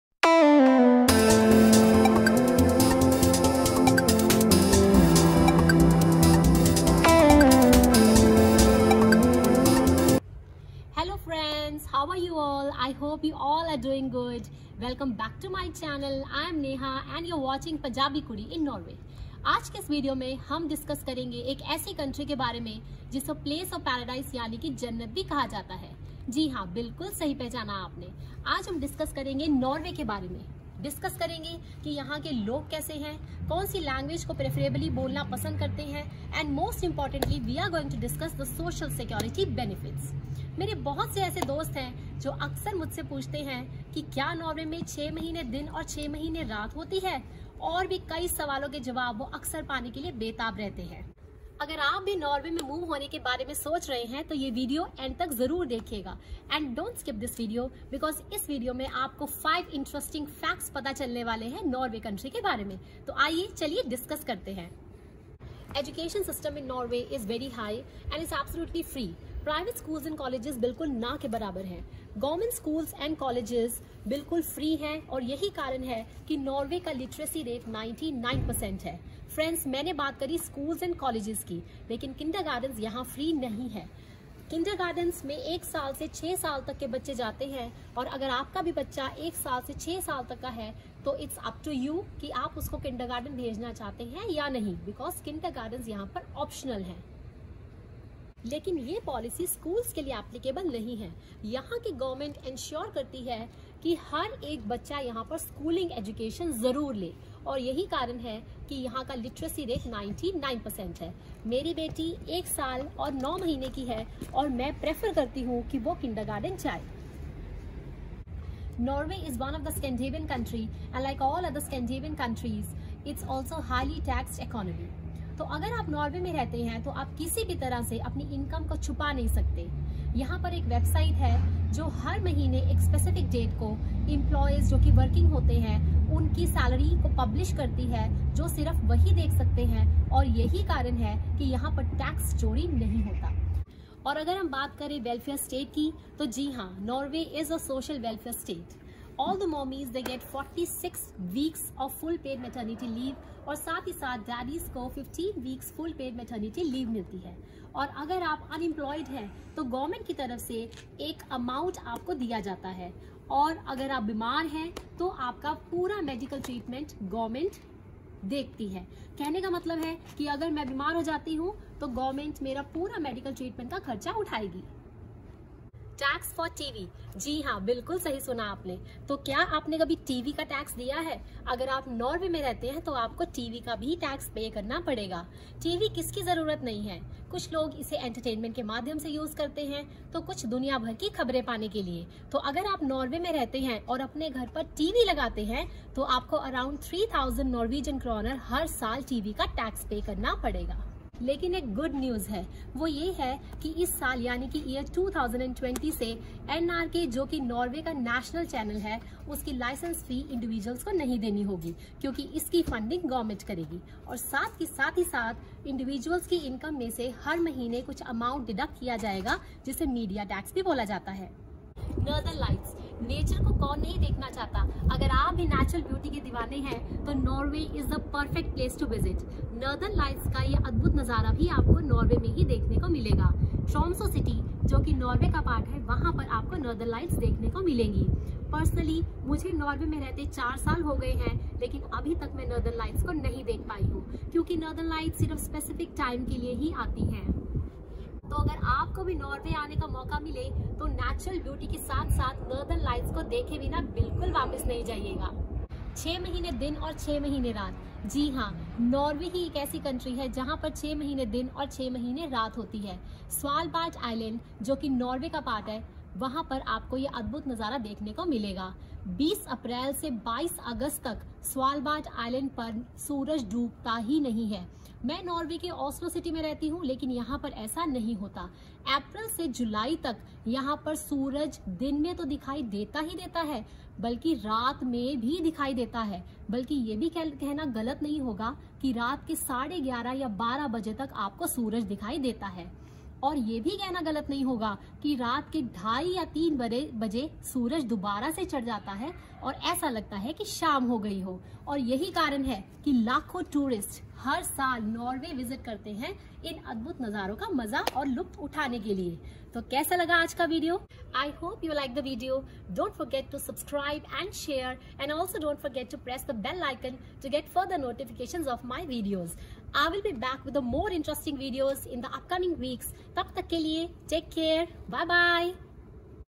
Incredible Places to Visit In Norway You won`t believe Exist
Starting from Beautiful city to Beautiful fjords and landscapes, Norway not only ends up with Northern lights but also is a country with 6 months day and 6 months night